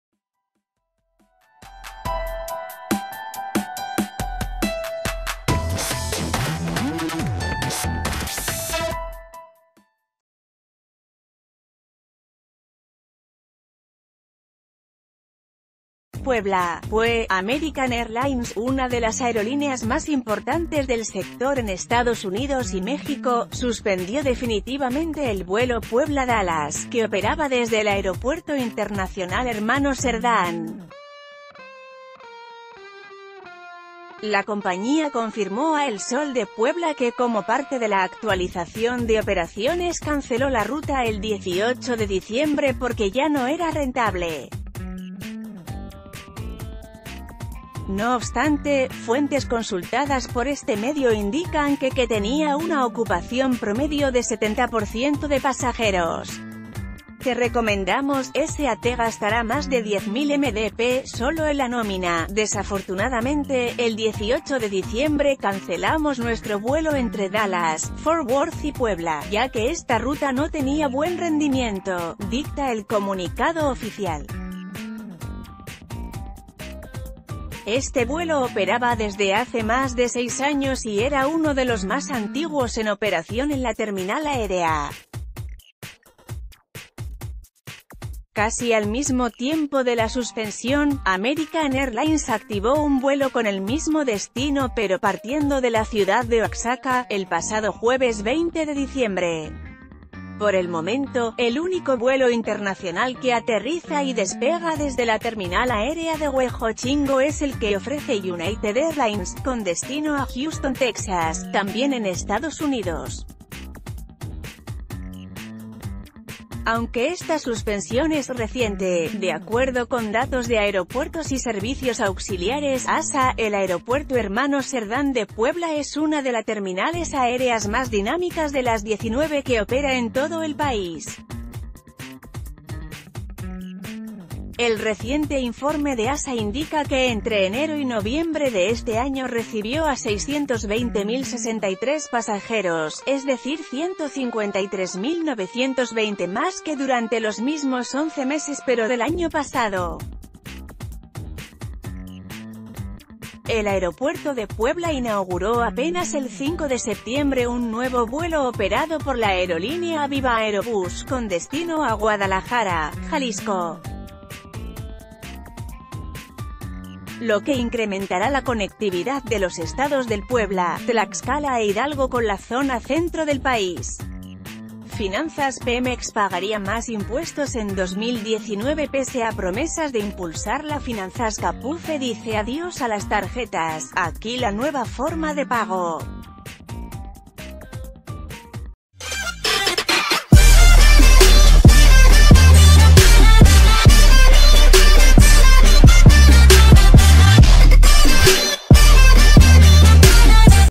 American Airlines cierra vuelo a Puebla
PUEBLA, Pue.- American Airlines, una de las aerolíneas más importantes del sector en Estados Unidos y México, suspendió definitivamente el vuelo Puebla-Dallas, que operaba desde el Aeropuerto Internacional Hermanos Serdán. La compañía confirmó a El Sol de Puebla que como parte de la actualización de...